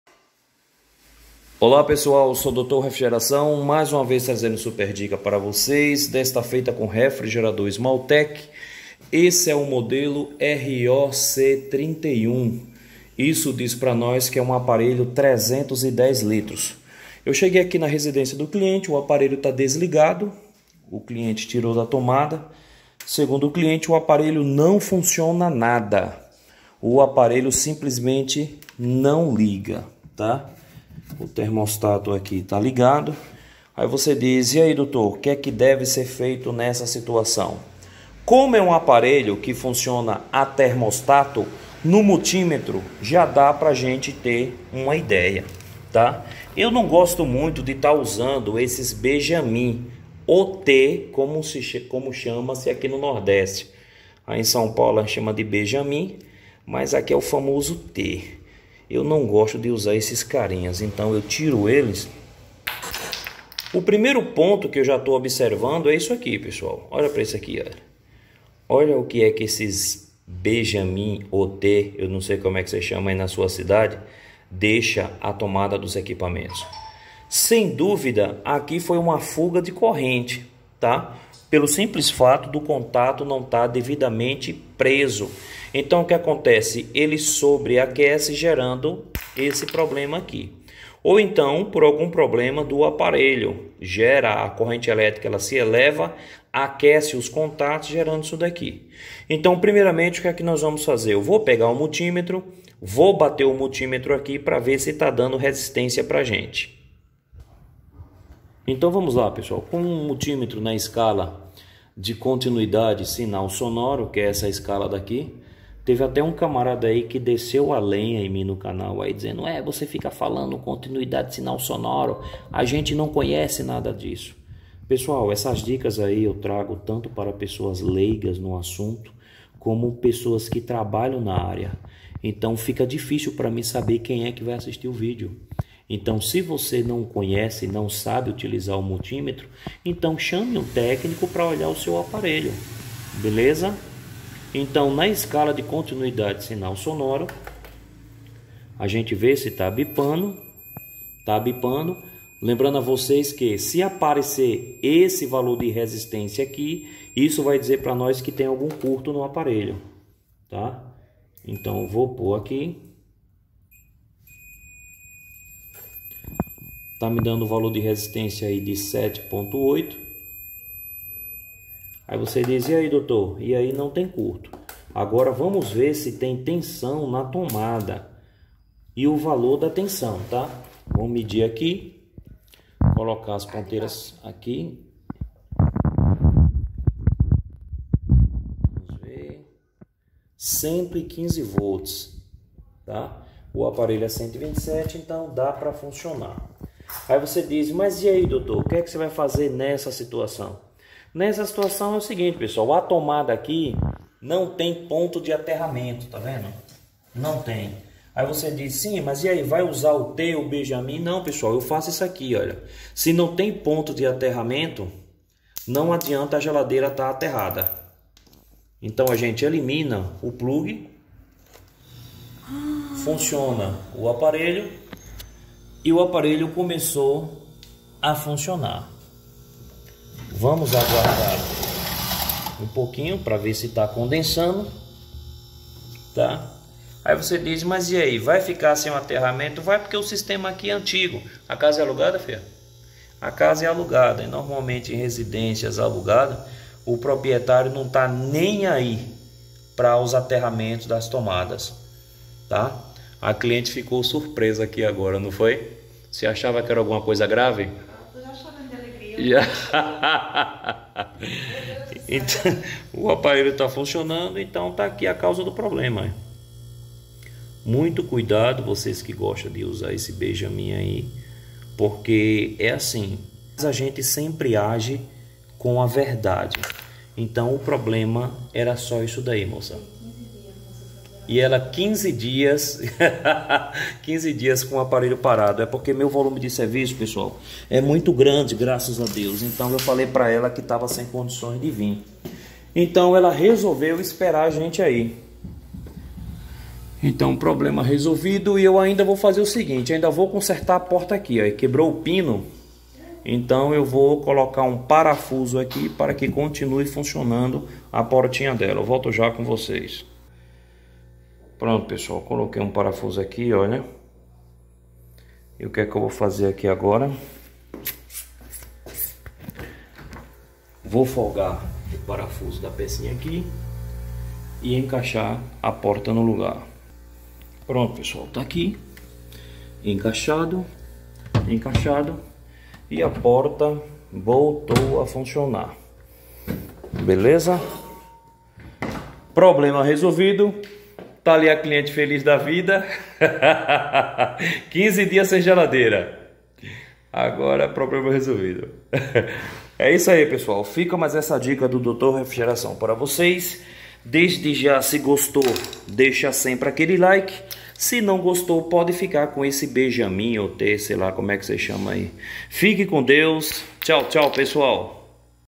GELADEIRA ESMALTEC, NÃO LIGA , NÃO FUNCIONA NADA.
NORMALMENTE PEGO PESSOAS USANDO ESSE ACESSÓRIO NO PLUG DA TOMADA.